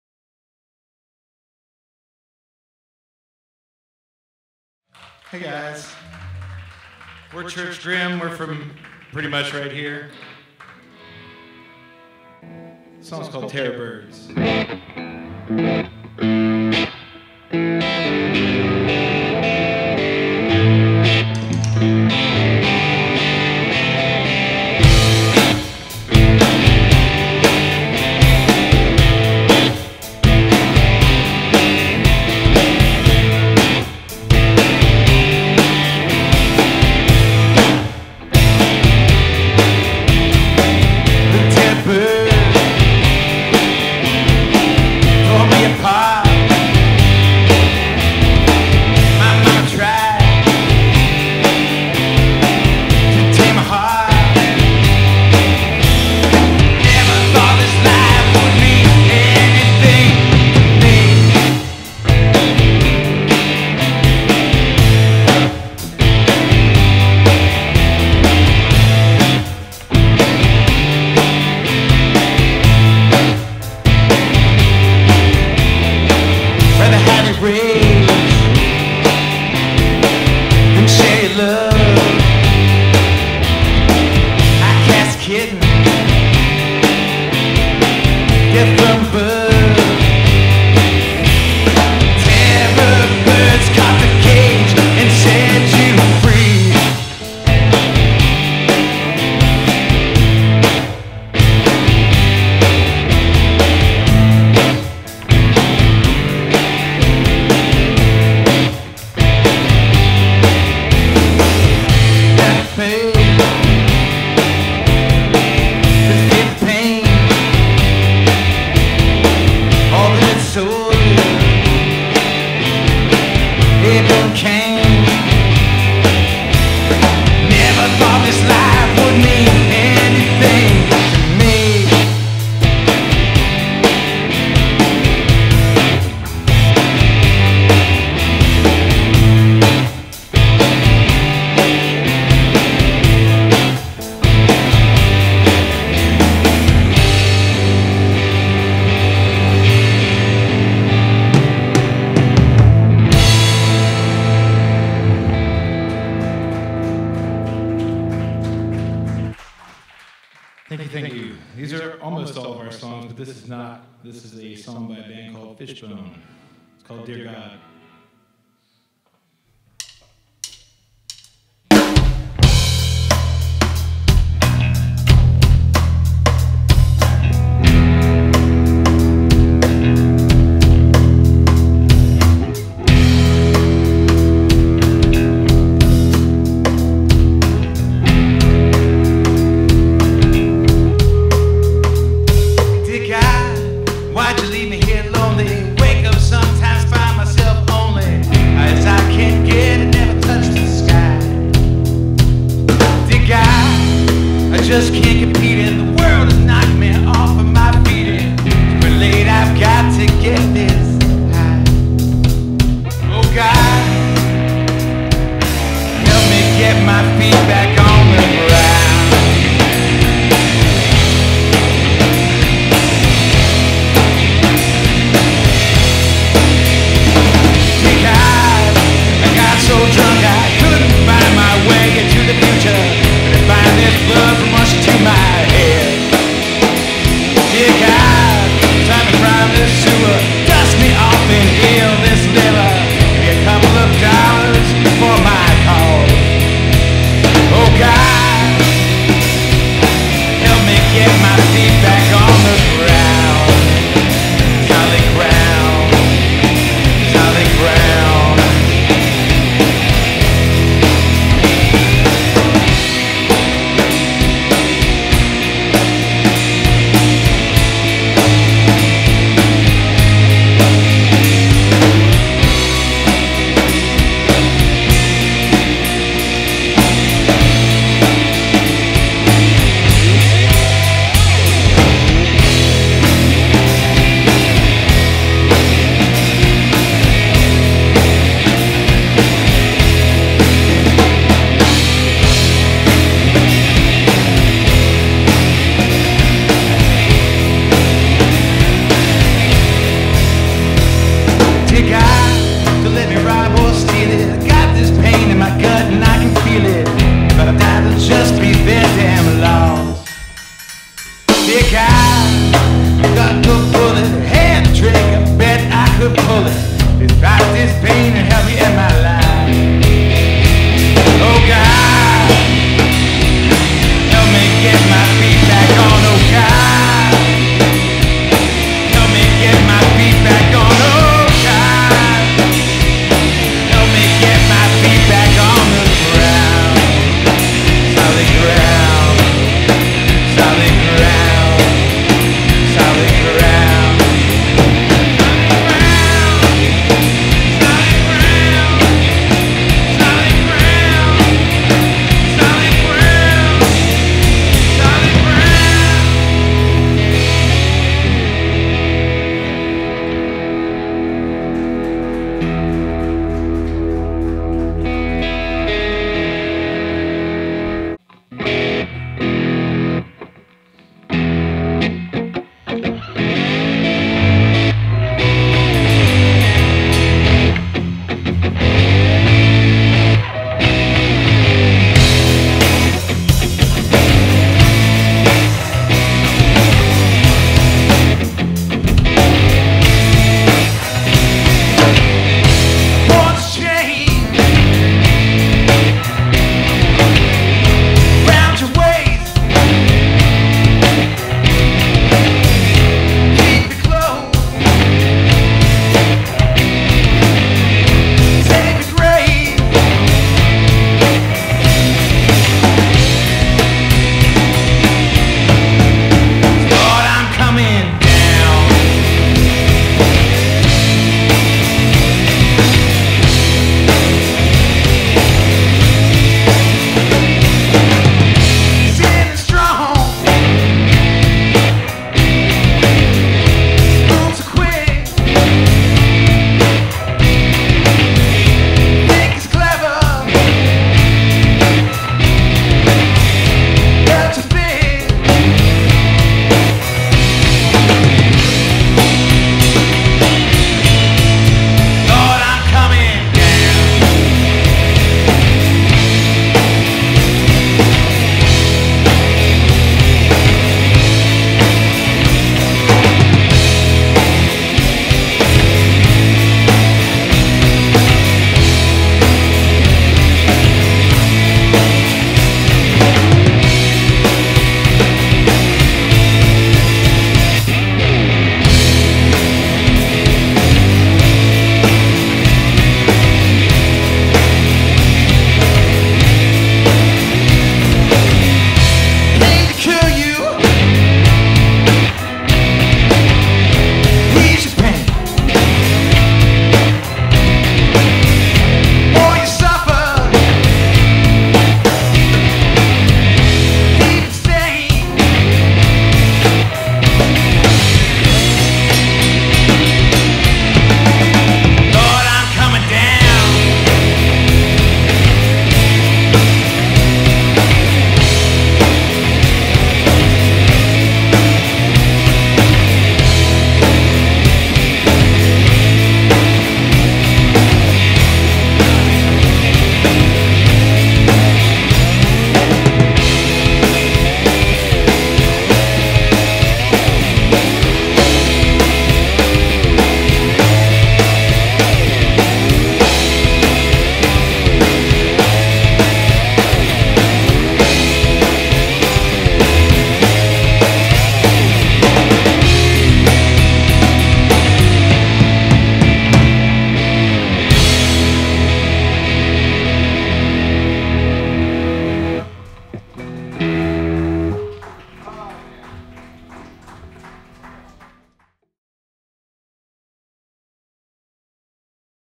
Church Grim | Live @ The Blue Crab | 05.11.19
A few songs from the show on 05.11.19 :)!